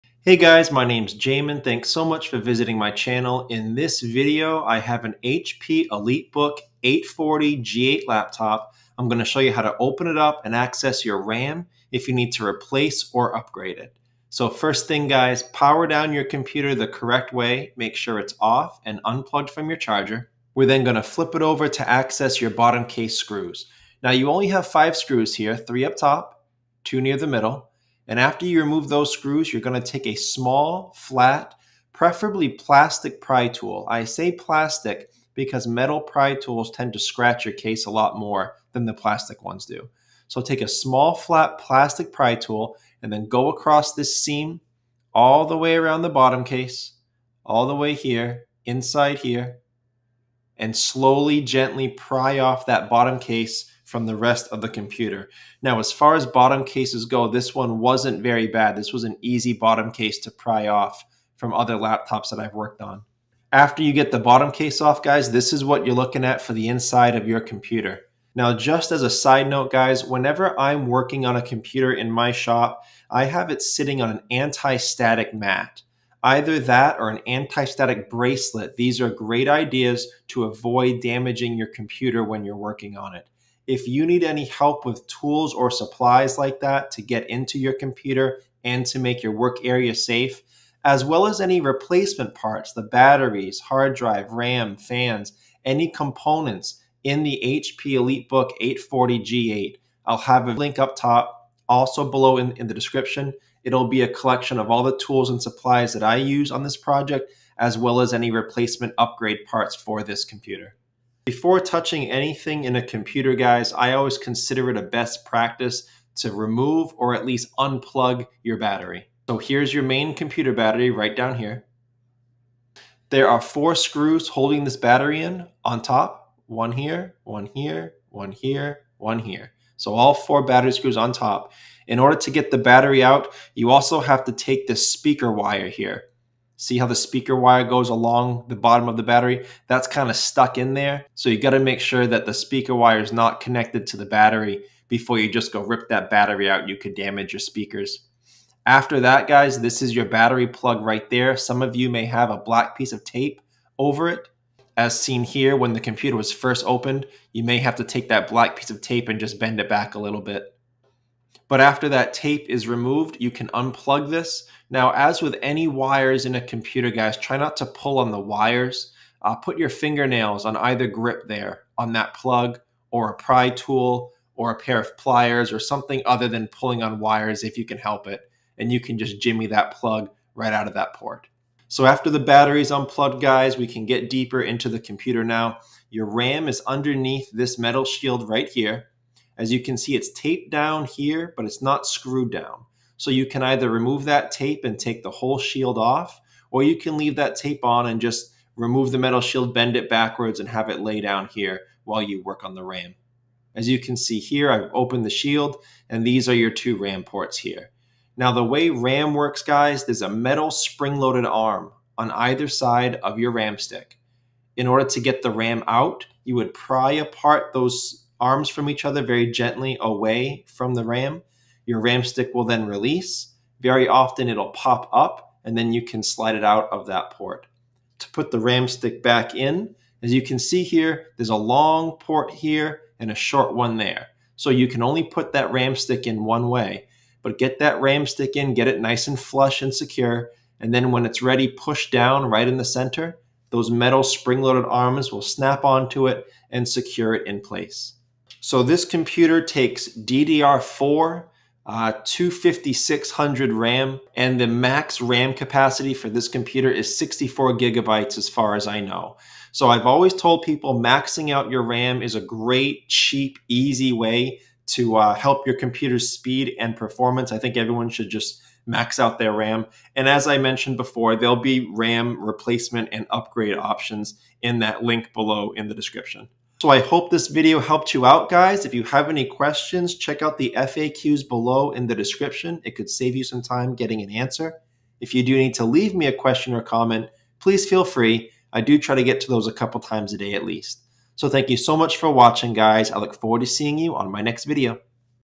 How To Replace Upgrade RAM - HP Elitebook 840 G8 Laptop Computer
In this video, I will show you how to safely access the RAM (Memory) in your HP Elitebook 840 G8 laptop computer.  Note: If you desire more speed / performance, consider upgrading the storage as well.
Elitebook 840 G8 Tools & Replacement Parts

FAQ's:
Battery Info:  Model CC03XL. HP Part Number: L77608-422
RAM Info:  Max 64GB,  Supports DDR4-PC4 SODIMM,  Runs at 3200MHz  (PC4-25600)